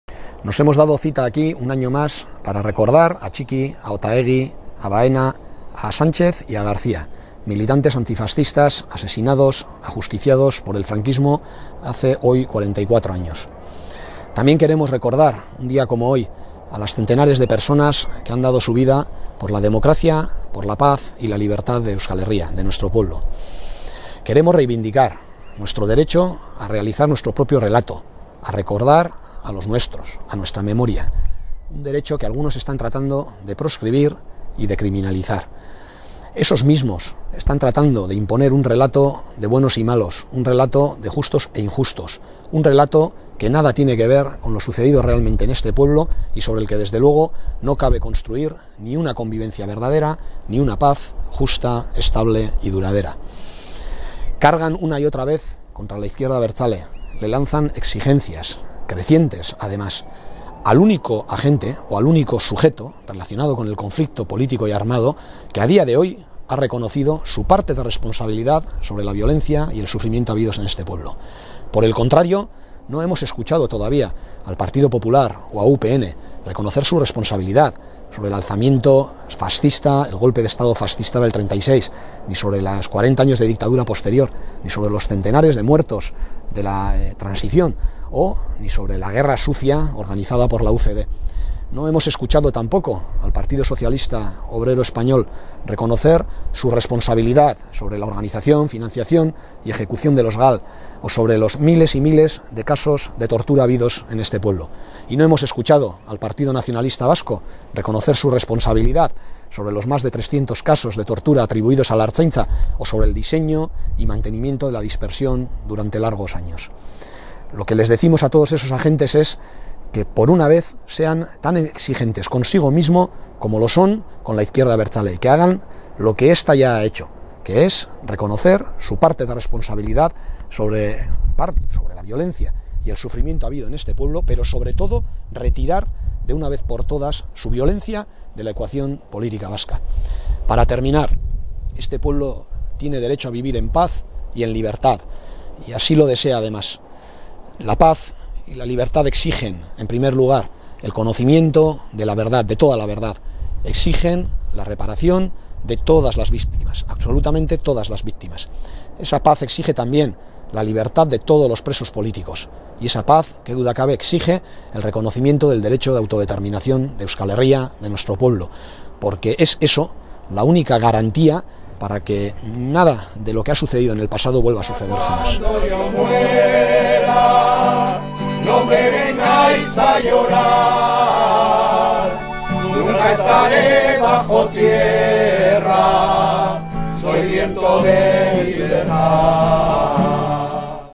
Declaraciones | ARKAITZ RODRIGUEZ, 44 años después del fusilamiento de Txiki y Otaegi
Frankismoak Txiki, Otaegi eta FRAPeko hiru kide fusilatu zituen orain 44 urte. Bakea eskatzen jarraitzen dugu. Horretarako zera behar da:
-Egia guztiaren ezagutza
-Biktima guztien errekonozimendu/erreparazioa
-Preso politiko guztien kaleratzea
-Autodeterminazio eskubidea onartzea